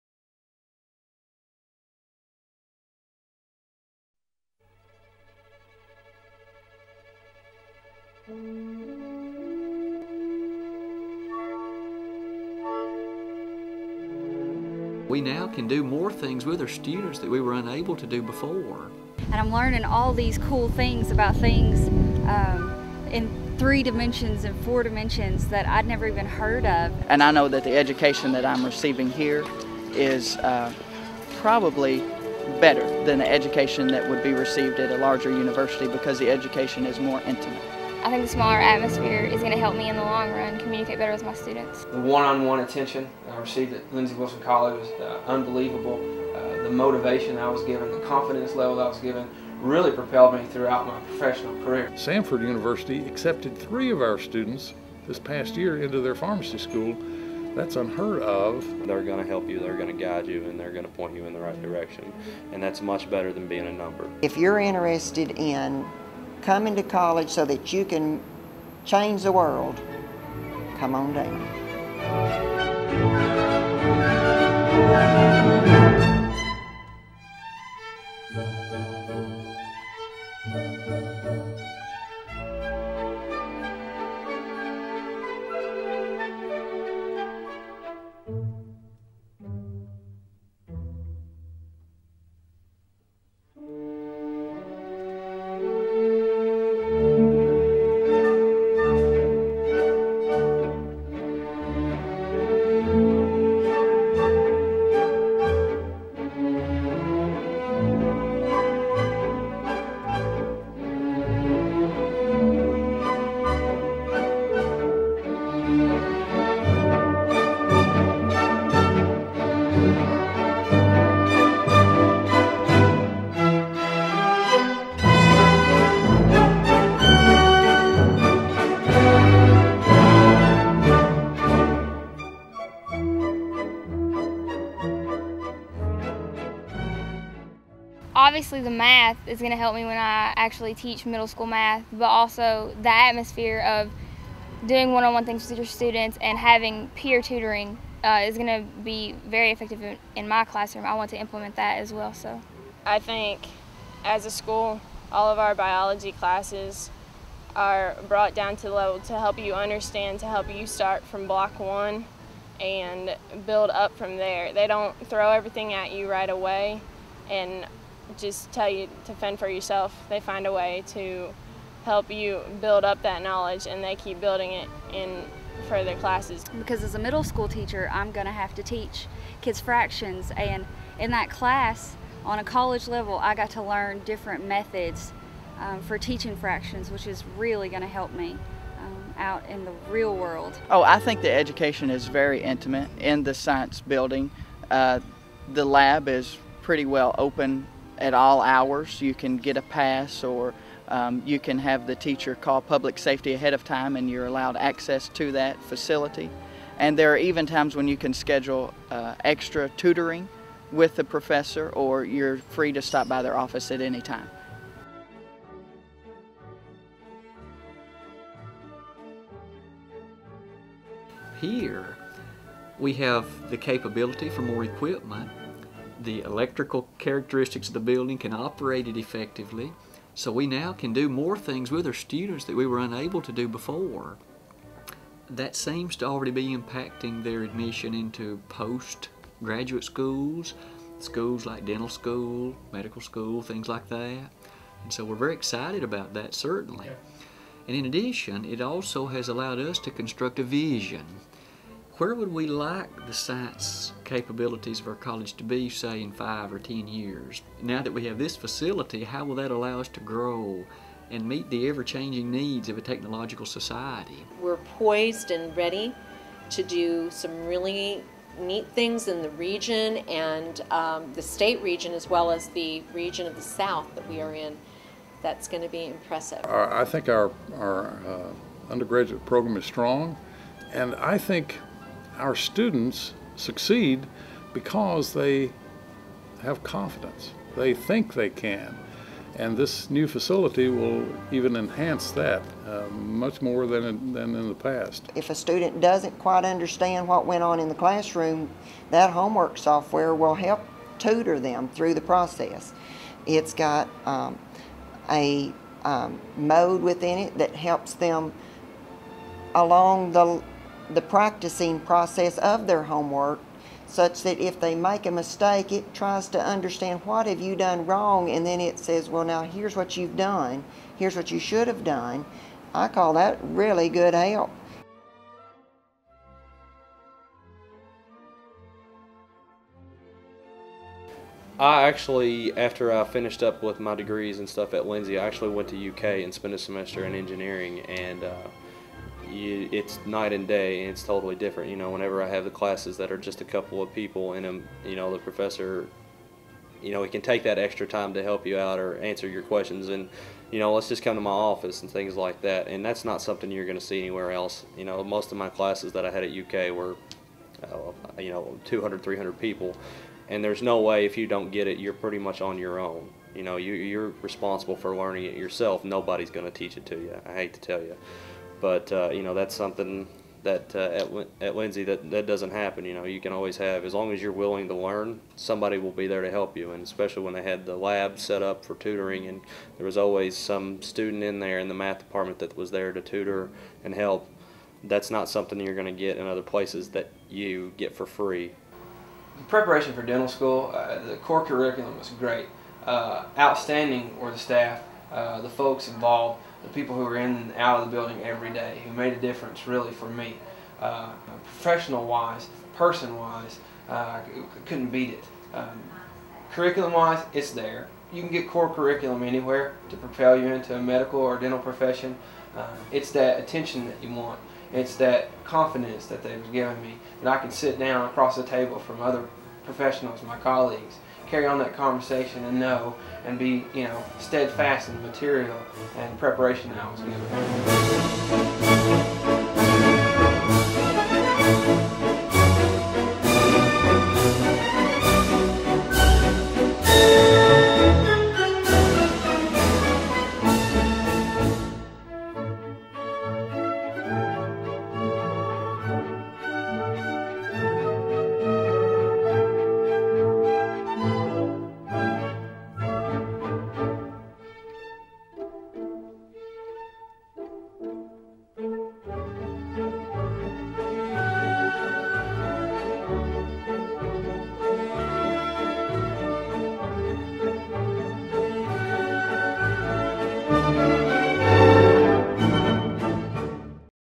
The Jim and Helen Lee Fugitte Science Center
A brief overview of how students at Lindsey Wilson College benefit from math and science programs at the Fugitte Science Center